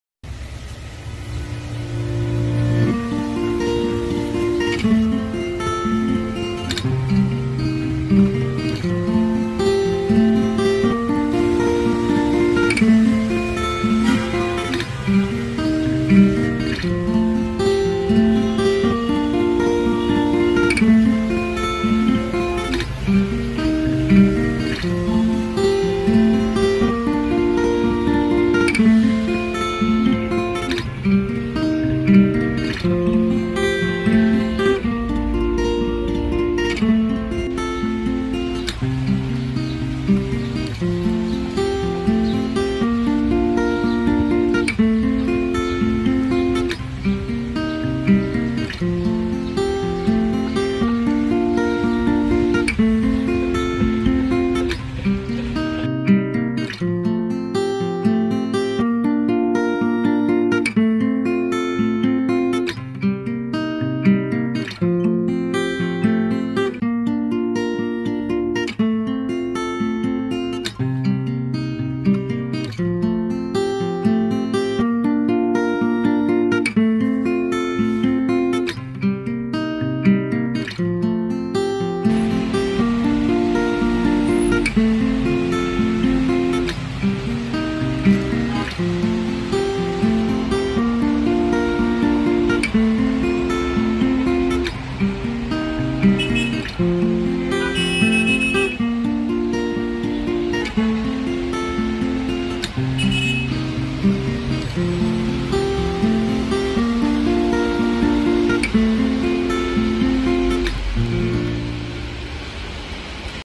Spring rain in Ulaanbaatar, Mongolia 🇲🇳 ♥️
Footage shot right outside of my rental property. Spring rain 🌧